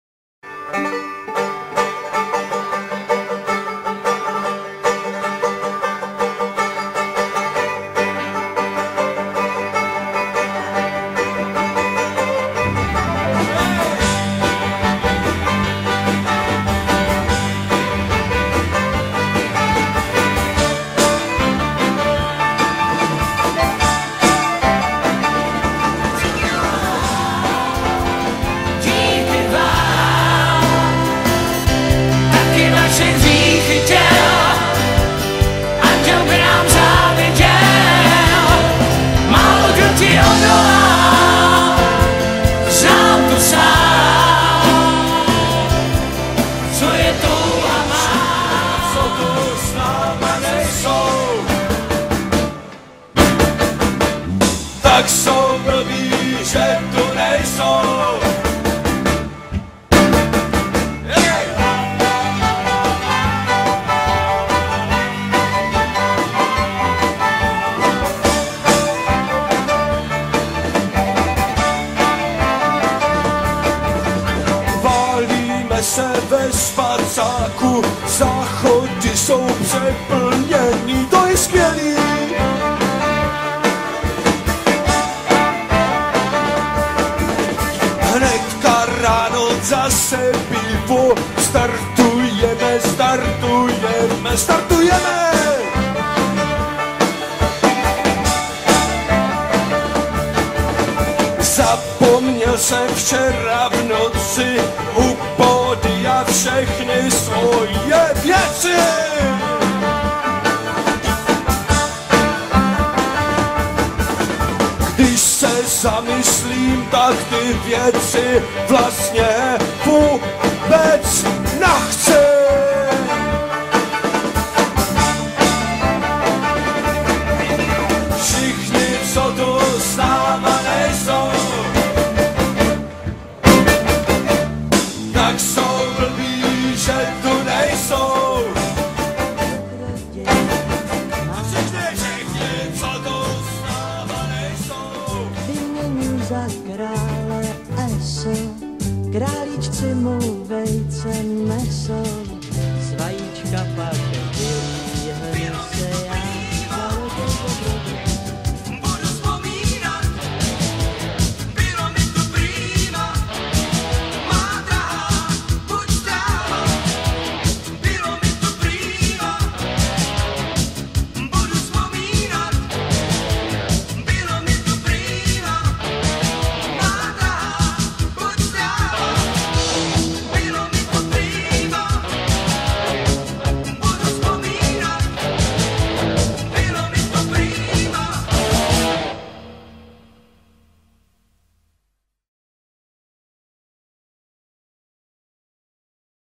Film Benátská Noc 2012
Benátská Noc v areálu vesec-Liberec s kapelou Wentyl a kamarády